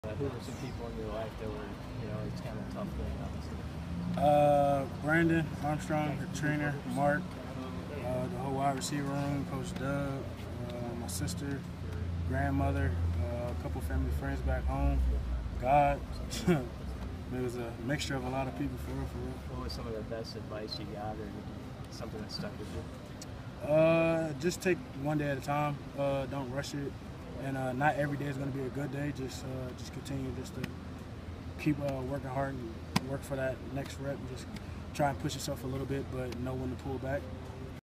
The best advice De'Mornay Pierson-El received during his recovery
Nebraska wide receiver De'Mornay Pierson-El lists the people in his life that he leaned on during his knee rehab and the best advice he got from those people after the Huskers first practice of fall camp.